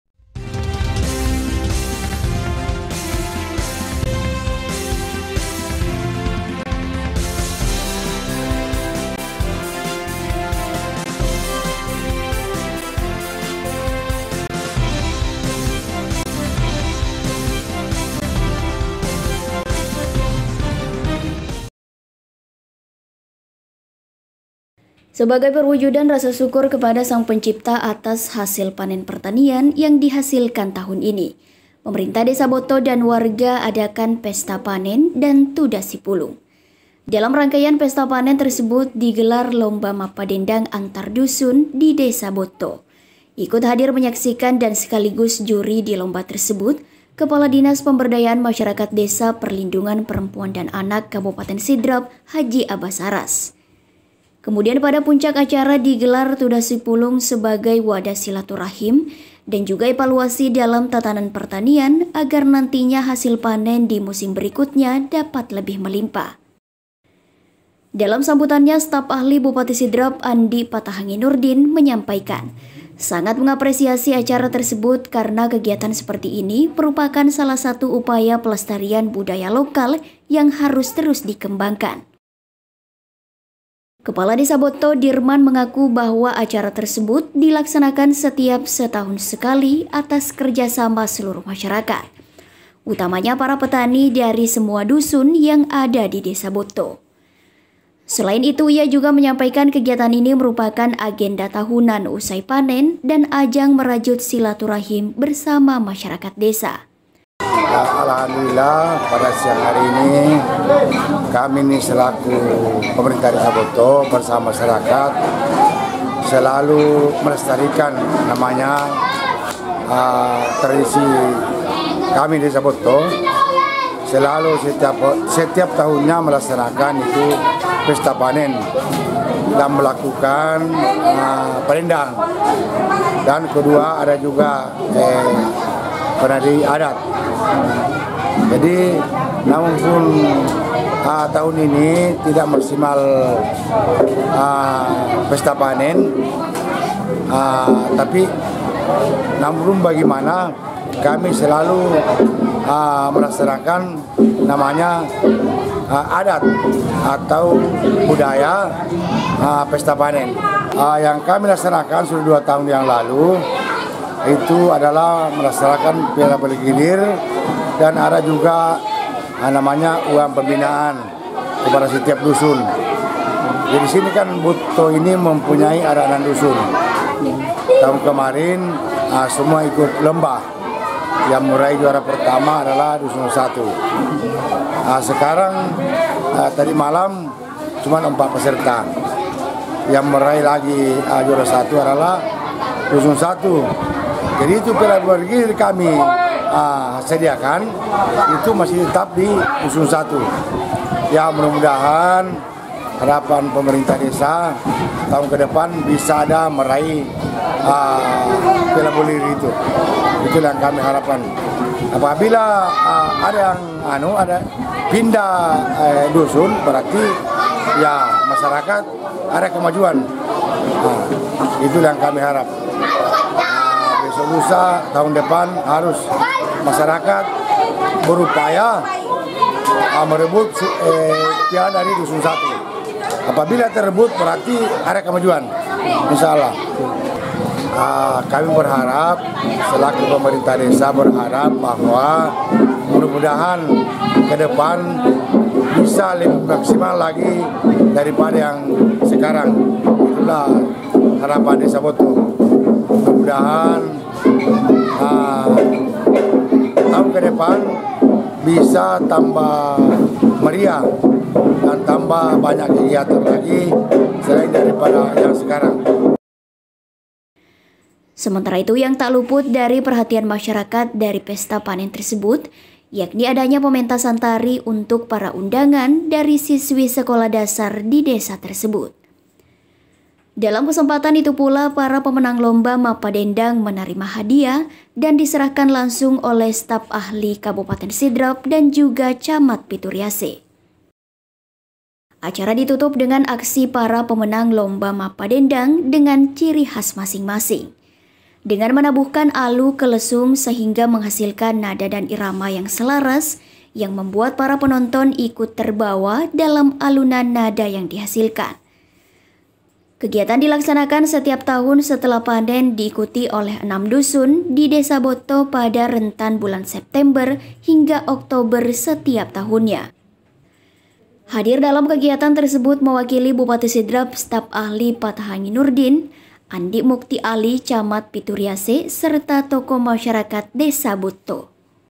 PESTA PANEN & TUDANG SIPULUNG DESA BOTTO PITU RIASE
MASYARAKAT DESA BOTTO GELAR TUDANG SIPULUNG DAN PESTA PANEN
SEBAGAI PERWUJUDAN RASA SYUKUR KEPADA SANG PENCIPTA ATAS HASIL PERTANIAN YANG DIHASILKAN TAHUN INI PEMERINTAH DESA BOTTO DAN WARGA ADAKAN PESTA PANEN DAN TUDANG SIPULUNG.
DALAM RANGKAIAN PESTA PANEN TERSEBUT DI GELAR LOMBA MAPPADENDANG ANTAR DUSUN DI DESA BOTTO/IKUT HADIR MENYAKASIKAN DAN SEKALIGUS JURI DI LOMBA TERSEBUT  KEPALA DINAS PEMBERDAYAAN MASYARAKAT DESA PERLINDUNGAN PEREMPUAN DAN ANAK  KABUPATEN SIDRAP H.ABBAS ARAS//
KEMUDIAN PADA PUNCAK ACARA DI GELAR TUDANG SIPULUNG SEBAGAI WADAH SILATURAHIM DAN JUGA EVALUASI DALAM TATANAN PERTANIAN AGAR NANTINYA HASIL PANEN DI MUSIM BERIKUTNYA DAPAT LEBIH MELIMPAH//
SB: ANDI PATAHANGI NURDI( STAF AHLI BUPATI SIDRAP)
DALAM SAMBUTANNYA STAF AHLI BUPATI SIDRAP ANDI PATAHANGI NURDIN MENYAMPAIKAN SANGAT MENGAPRESIASI ACARA TERSEBUT/KARENA KEGIATAN SEPERTI INI MERUPAKAN SALAH SATU UPAYA PELESTARIAN BUDAYA LOKAL YANG HARUS TERUS DI KEMBANGKAN
SB: KEPALA DESA BOTTO DIRMAN
KEPALA DESA BOTTO DIRMAN , MENGAKU, BAHWA ACARA TERSEBUT DILAKSANAKAN SETIAP SETAHUN SEKALI ATAS KERJASAMA SELURUH MASYARAKAT, UTAMANYA PARA PETANI DARI SEMUA DUSUN YANG ADA DI DESA BOTTO
SELAIN ITU, IA JUGA MENYAMPAIKAN KEGIATAN INI MERUPAKAN AGENDA TAHUNAN USAI PANEN DAN AJANG MERAJUT SILATURAHIM BERSAMA MASYARAKAT DESA
SEMENTARA ITU YANG TAK LUPUT DARI PERHATIAN MASYARAKAT DARI PESTA PANEN TERSEBUT YAKNI ADANYA PEMENTASAN TARI UNTUK PARA UNDANGAN DARI SISWI SEKOLAH DASAR DI DESA TERSEBUT/
DALAM KESEMPATAN ITU PULA PARA PEMENANG LOMBA MAPPADENDANG MENERIMA HADIAH DAN DI SERAHKAN LANGSUNG OLEH STAF AHLI KABUPATEN SIDRAP DAN JUGA CAMAT PITU RIASE//
ACARA DITUTUP DENGAN AKSI PARA PEMENANG LOMBA MAPPADENDANG DENGAN CIRI KHAS MASING MASING DENGAN MENABUHKAN ALU KE LESUNG SEHINGGA MENGHASILKAN NADA DAN IRAMA YANG SELARAS YANG MEMBUAT PARA PENONTON IKUT TERBAWA DALAM ALUNAN NADA YANG DI HASILKAN///
KEGIATAN DILAKSANAKAN SETIAP TAHUN SETELAH PANEN, DIIKUTI OLEH 6 DUSUN DI DESA BOTTO PADA RENTAN BULAN SEPTEMBER HINGGA OKTOBER SETIAP TAHUNNYA
HADIR DALAM KEGIATAN TERSEBUT, MEWAKILI BUPATI SIDRAP STAFF AHLI PATAHANGI NURDIN ANDI MUKTI ALI,CAMAT PITU RIASE  SERTA TOKOH MASYARAKAT DESA BOTTO

EP